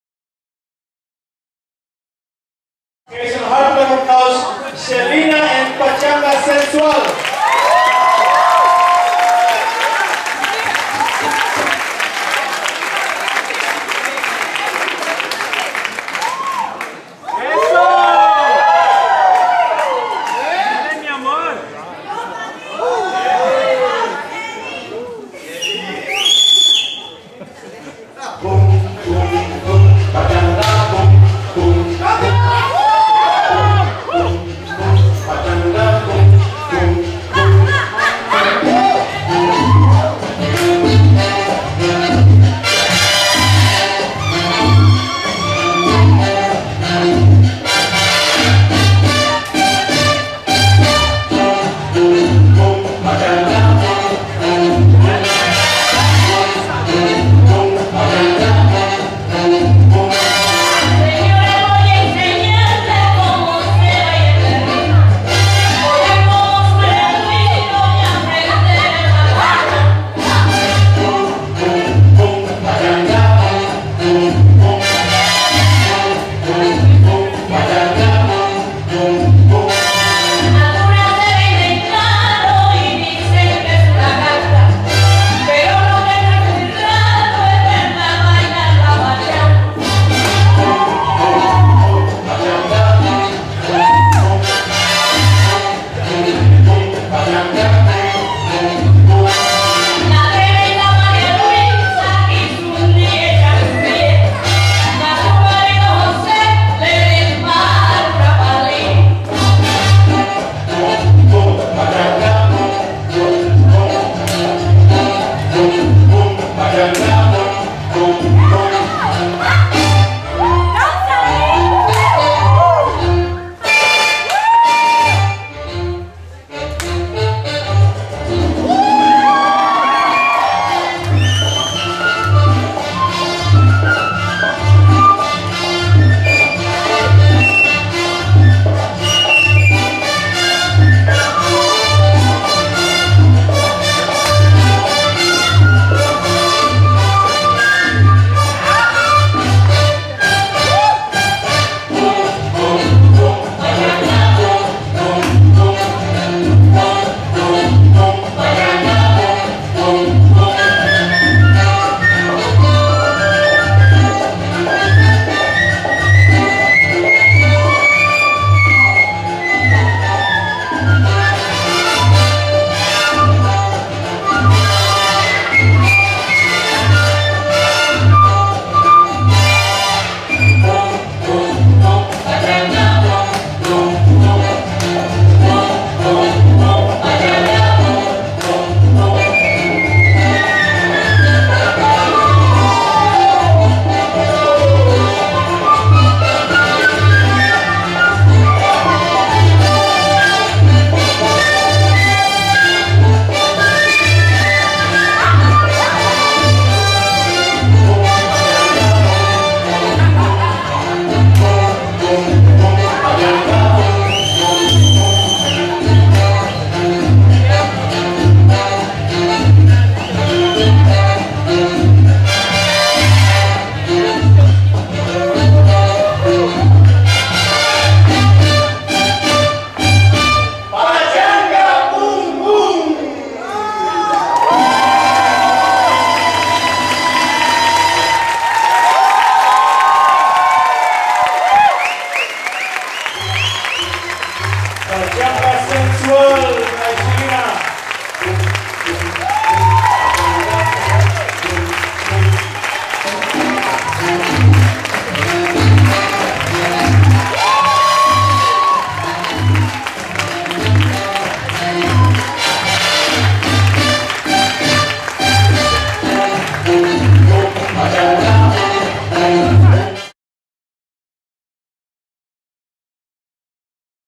Pachanga Sensual olv Shelina @ Salsa Casa Loca
Pachanga Sensual olv Shelina @ Salsa Casa Loca, Utrecht - NL, January 26th 2013

A-line Dance Videos, For all your HD Dance Videos